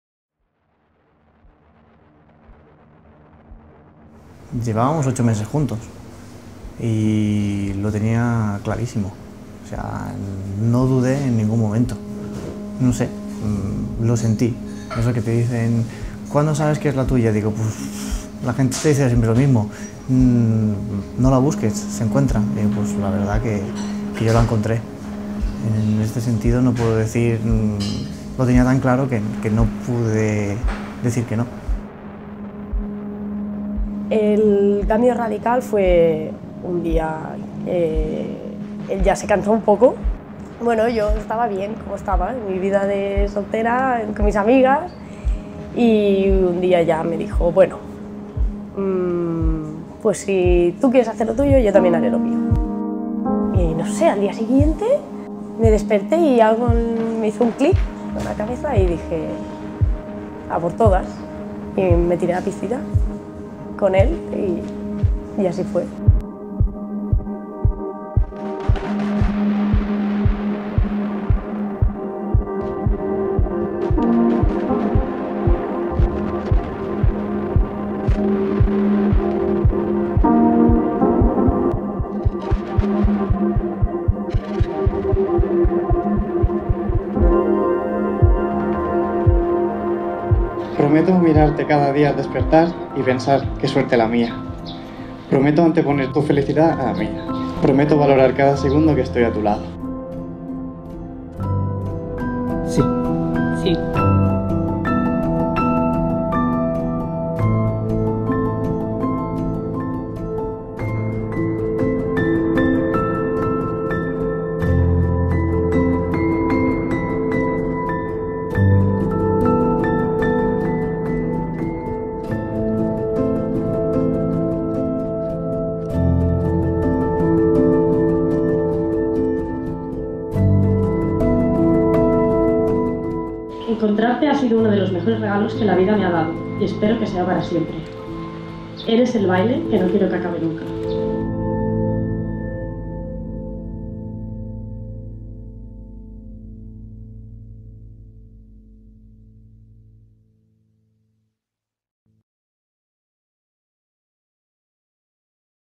Vídeo de boda en Mas Passamaner | Noemí + Iván
Para nosotros no son una pareja más (jamás ninguna lo fué), con ellos ganamos unos amigos y unos profesores de baile..

Ellos querían una FIESTA, y así fue. Todo el mundo bailaba, ¡Hasta nosotros!

Hacer un vídeo de boda en Mas Passamaner con ellos fue muy fácil!